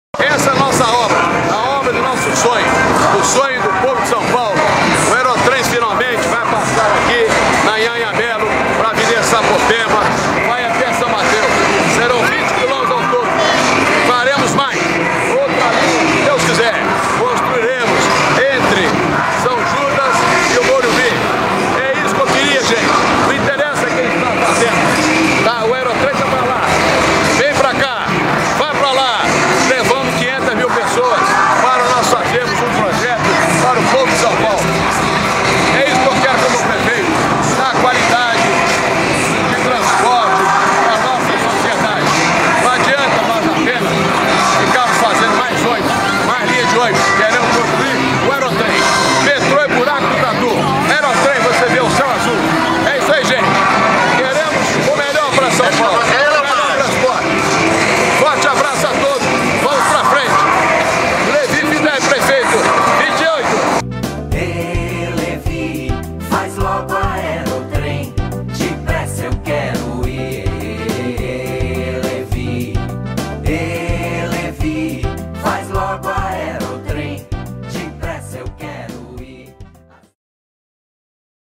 Aerotrem em São Paulo, veja o que Levy Fidelix sempre falou, começou a funcionar na Zona Leste.
LEVY FIDELIX TBT (32): Aerotrem em São Paulo, veja o que Levy Fidelix sempre falou, começou a funcionar na Zona Leste.